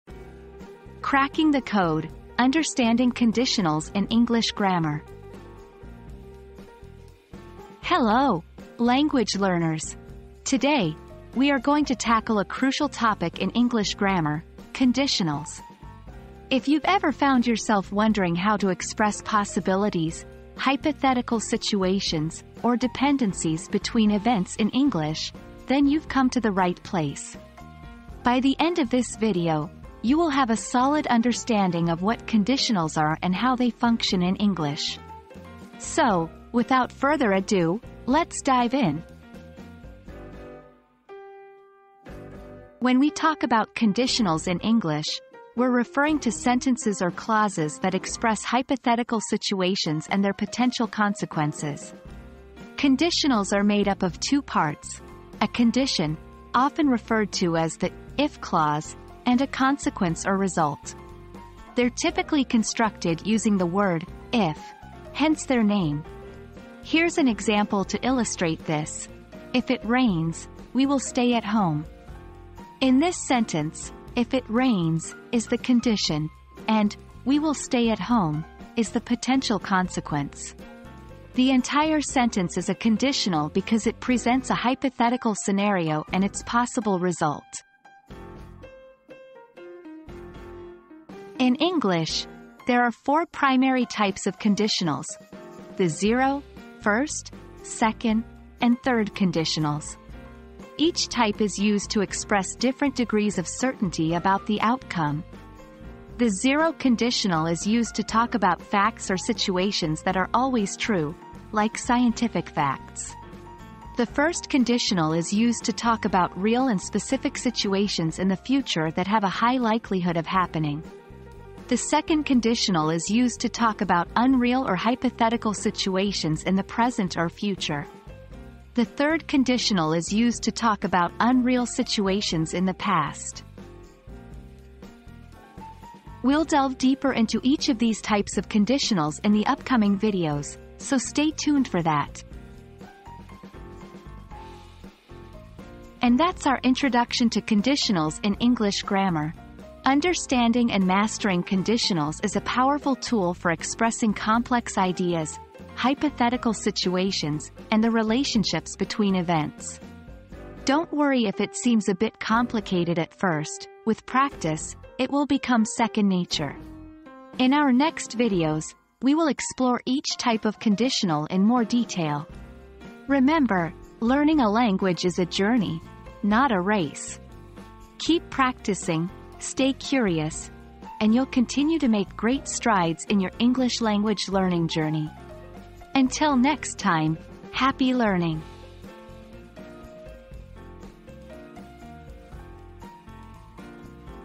Cracking the Code: Understanding Conditionals in English Grammar
Cracking the Code: Understanding Conditionals in English Grammar • Learn the ins and outs of conditionals in English grammar and unlock the key to expressing possibilities and hypothetical situations. Dive into the four types of conditionals and gain a solid understanding of how they function in English.

00:00 • Introduction - Cracking the Code: Understanding Conditionals in English Grammar
00:42 • Defining Conditionals
01:30 • The Four Types of Conditionals